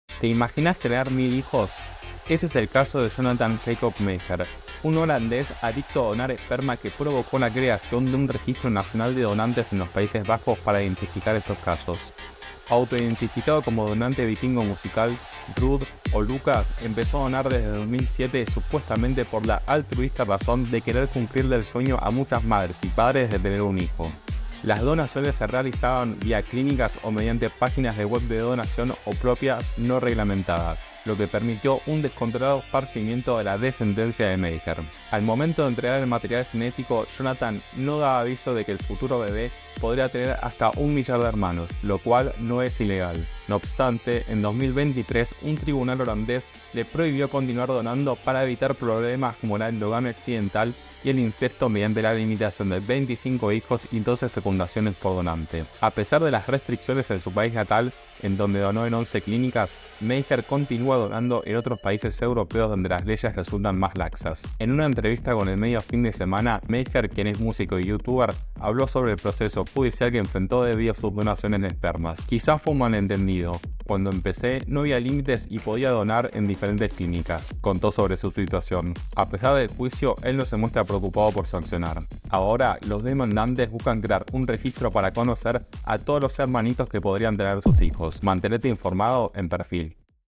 Donó esperma, tuvo 1.000 hijos y logró lo impensado
👶 Un neerlandés comenzó a donar esperma hace 17 años, alcanzando el millar de hijo y provocando que el país europeo tuviese que regular su actividad para evitar endogamia accidental.